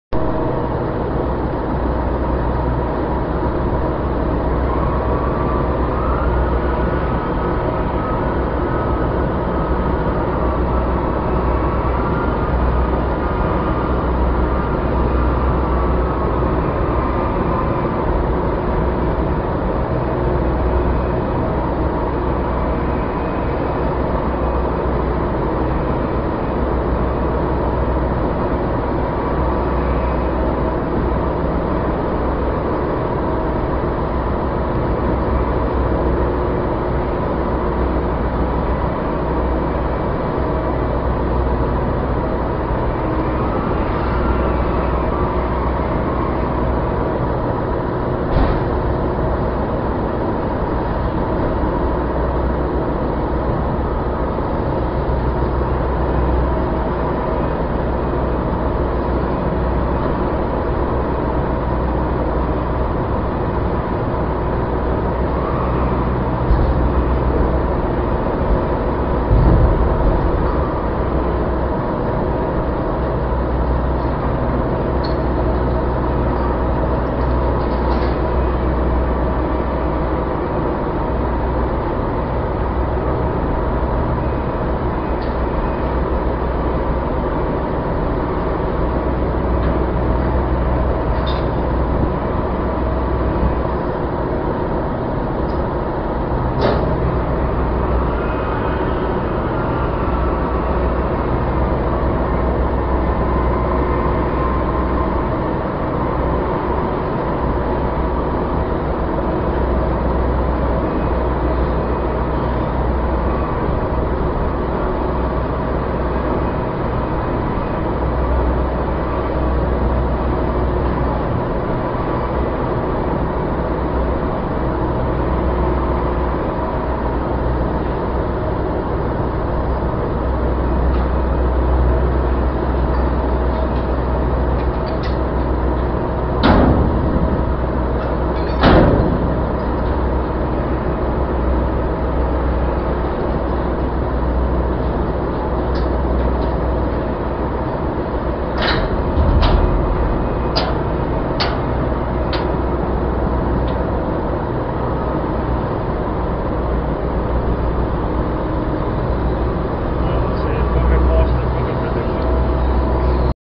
Working on a Supply Ship Deck
Me and Paul working (not very hard) on a supply ship in the North Sea.